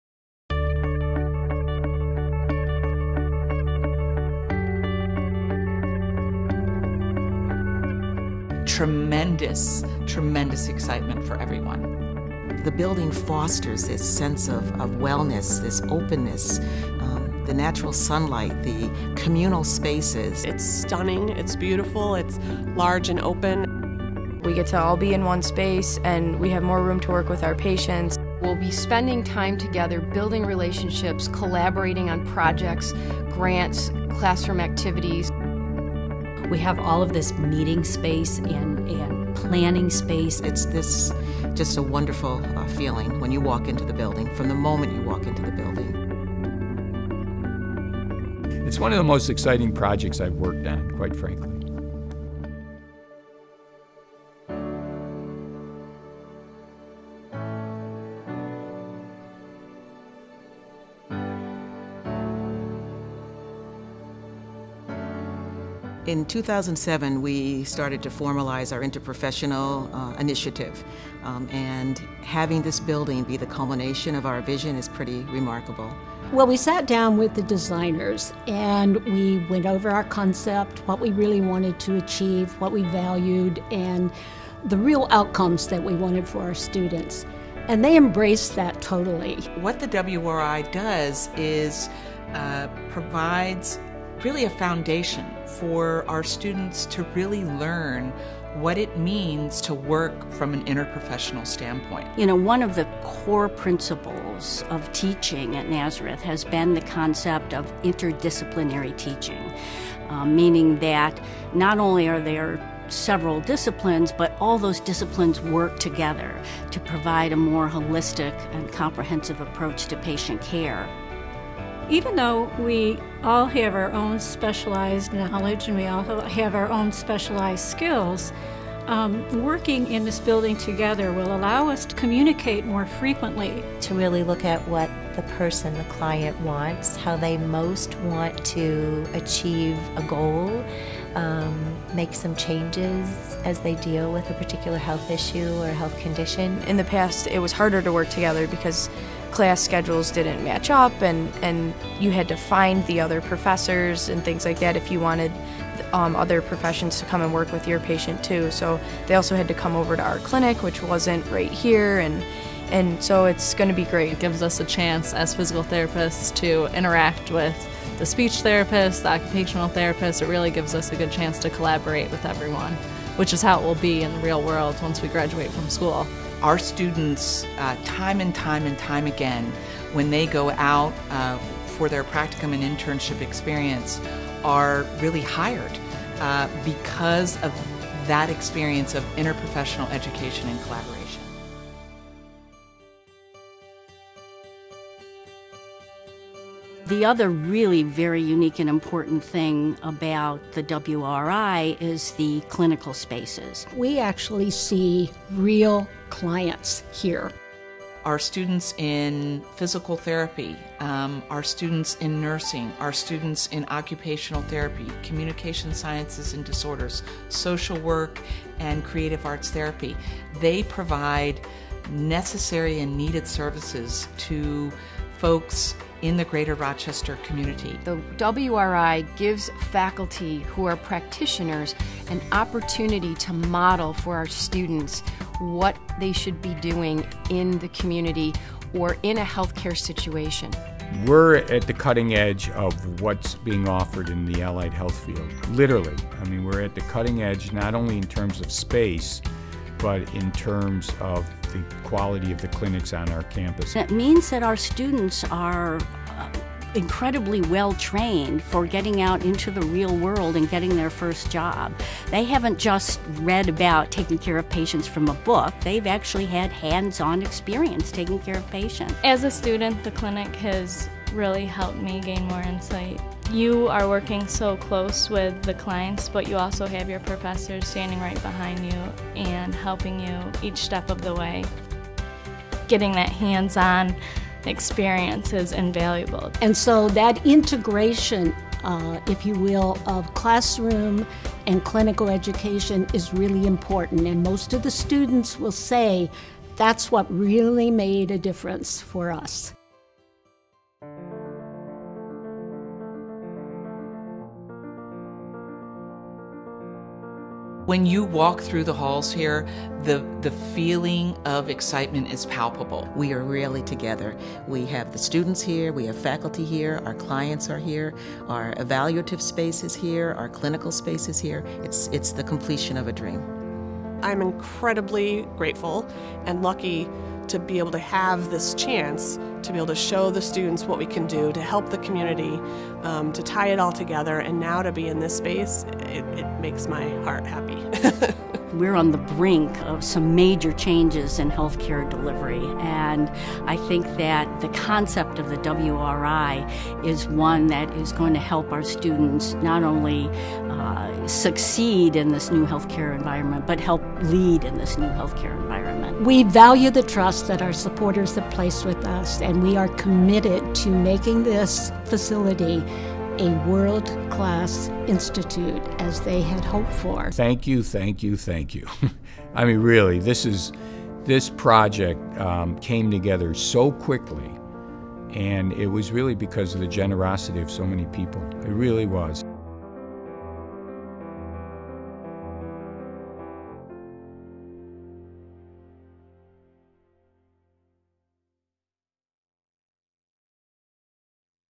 York Wellness and Rehabilitation Institute (Thank you!)
The York Wellness and Rehabilitation Institute doubles the School of Health and Human Services’ clinic and collaboration space, and provides a progressive interprofessional teaching and learning environment where students and faculty can collaborate in clinical services, research, and the development of innovative practices.